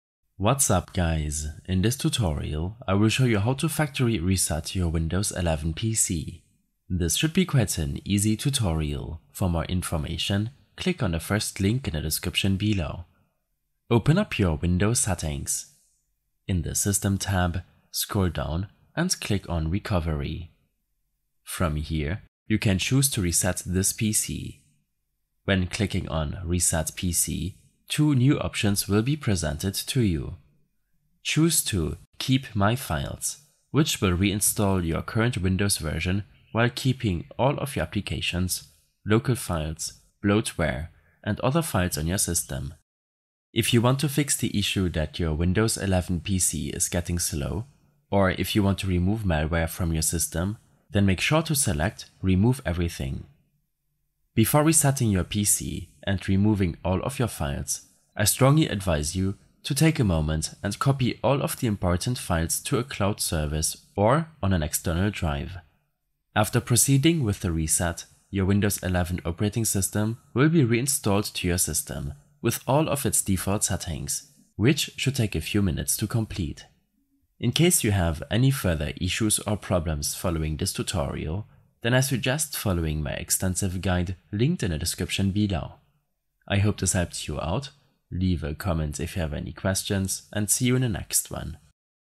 Factory Reset a Windows 11 PC - Easy Guide
A short tutorial on how to factory reset your Windows 11 PC.

Timestamps:
0:00 - Introduction
0:13 - Recovery Settings
0:25 - Choose To Keep Files
0:43 - Remove Everything